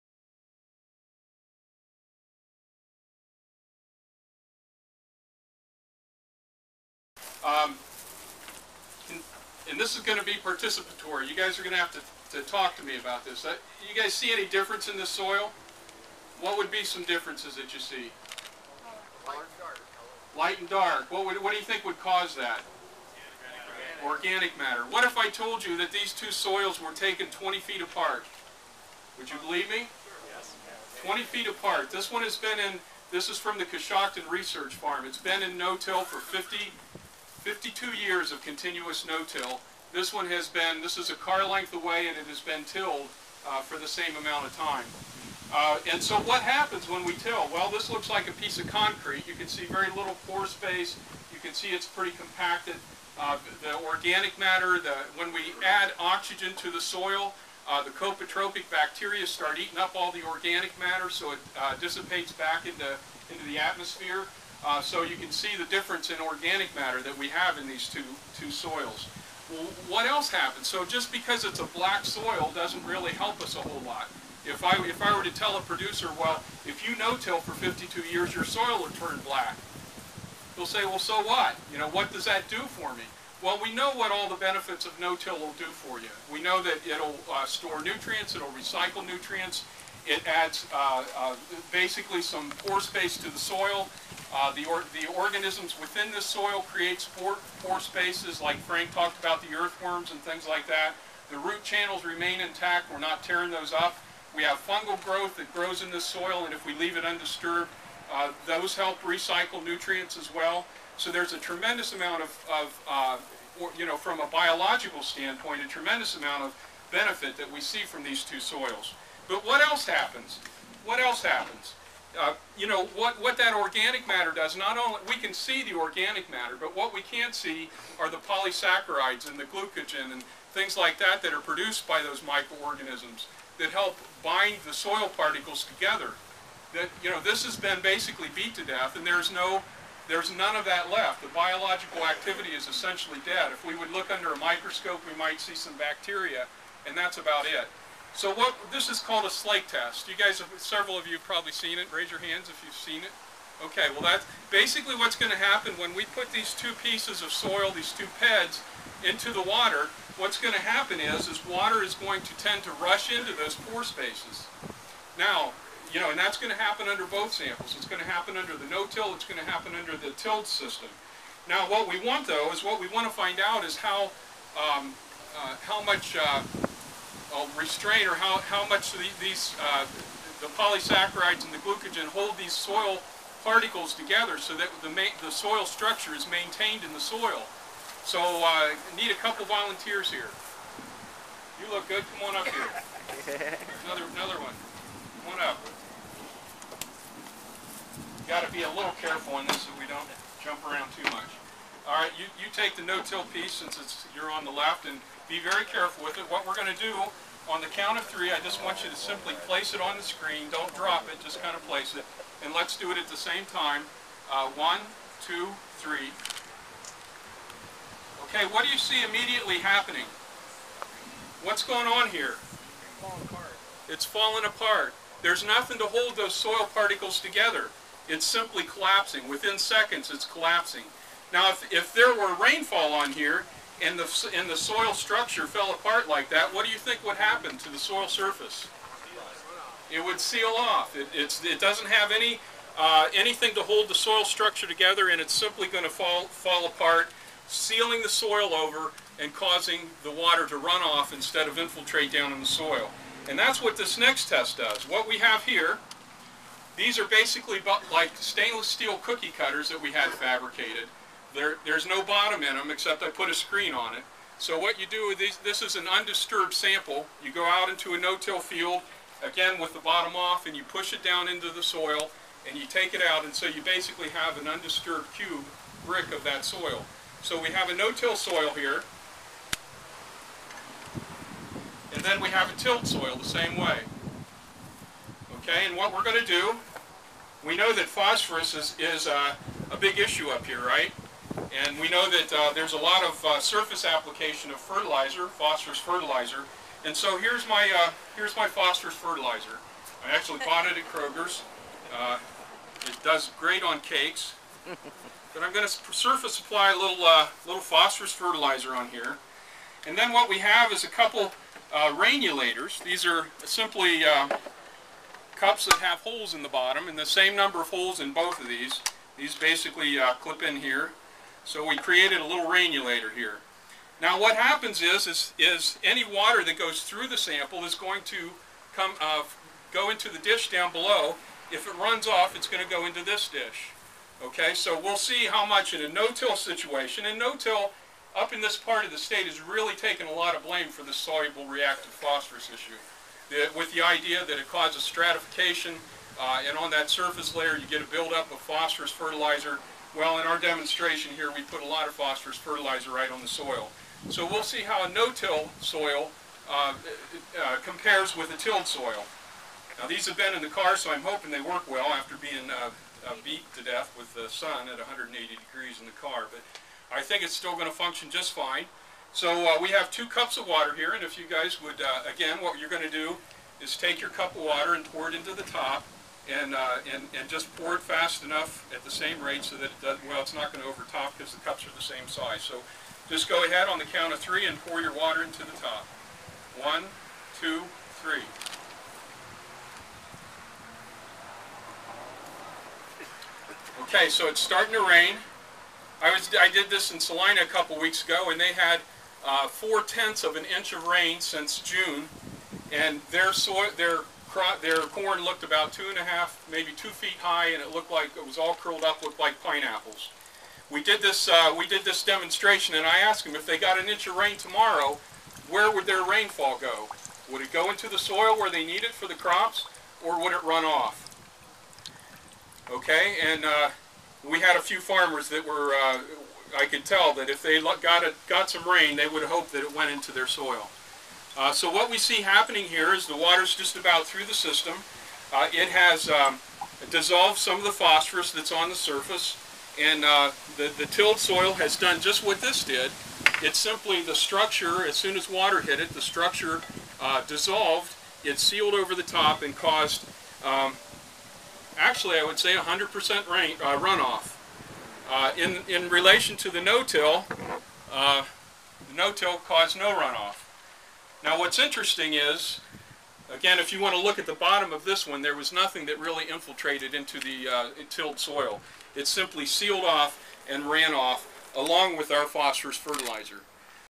Differences in Tilled and No Till Soils
An Experiment to show the differences in the soil structure of a Heavily Tilled Farm using chemical fertilizers and a Lightly Tilled Farm using natural organic fertilizers. The experiment show two samples of farms barely 20ft apart, but which show remarkably different behaviour in water and fertilizer retention.

In the short experiment shown in the video, it is conclusively shown that a lightly tilled farm using natural organic fertilizers is able to retain the natural bacteria and other living organisms thereby making the soil bind very closely. Due to this phenomenon, the lightly tilled soil is able to retain water and with it the fertilizers even in heavy rainfall. Whereas, the heavily tilled mechanized farm is not able to retain even a fraction of rainwater. This causes water run-off and soil erosion.

With this experiment, a case is made out for light tilling of farms supplemented with natural organic fertilizers. This is very relevant to our farming sector, where water retention and prevention of wasteful run-offs is crucial for farming.